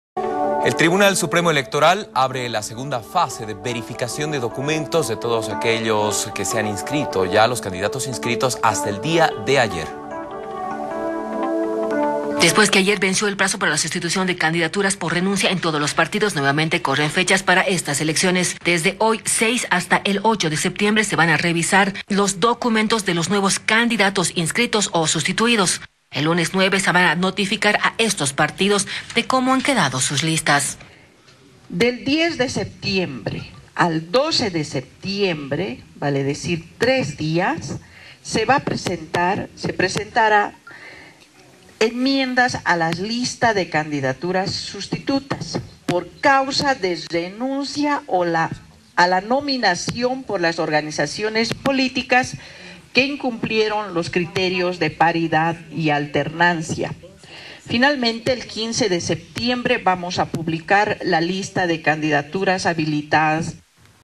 El 15 de septiembre saldrá la lista de habilitados
ATB – 3ra Edición
 El TSE habré la fase de verificación de documentos de los candidatos inscritos hasta el día de ayer. Desde hoy hasta el 8 de septiembre se van a revisar los documentos de los candidatos inscritos o sustituidos. El lunes 9 se va a notificar a estos partidos de como han quedado sus listas.